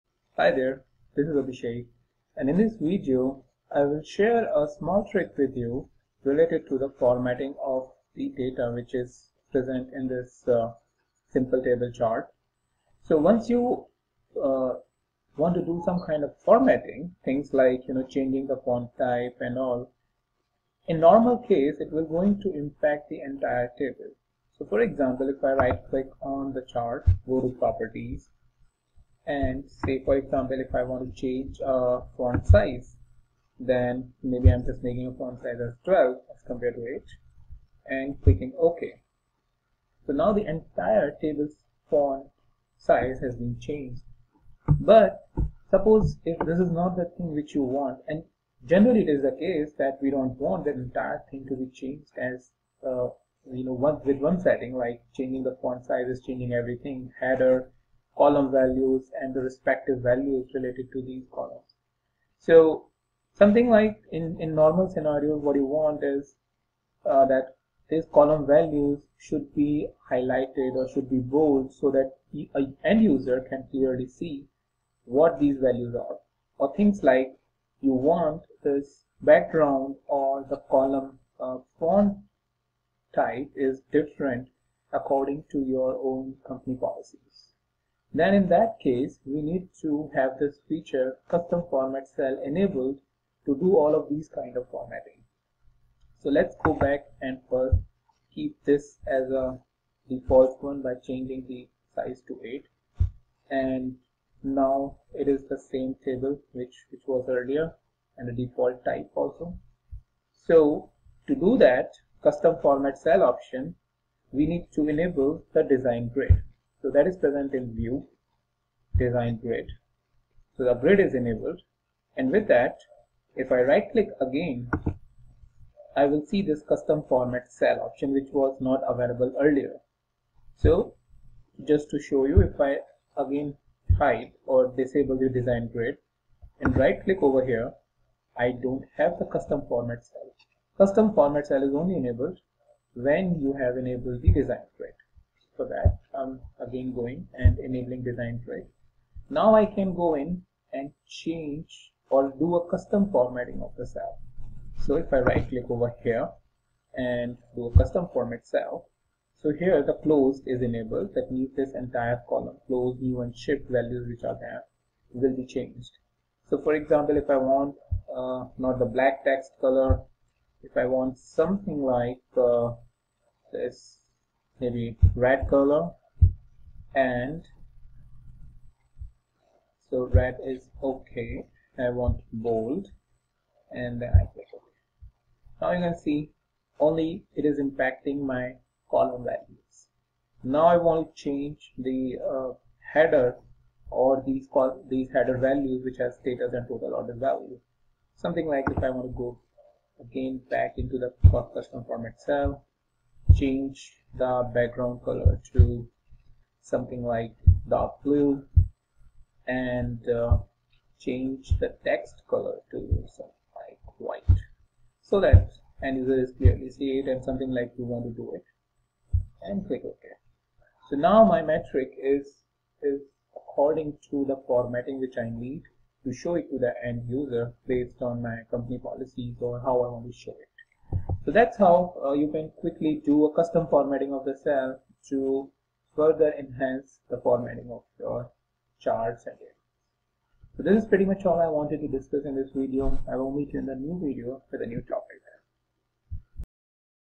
QlikView Tutorials | QlikView Tips and Tricks | Custom format cell in QlikView
Sometimes your charts are not formatted as per your needs and you are in need of something which can help you doing the custom formatting. Custom format cell is the option which you can use to do the formatting as per your needs. In this video I’ve shown how you can enable the custom formatting option and use it in your charts.

I hope you will find this video useful and an addition to your learning of QlikView.

-Abhishek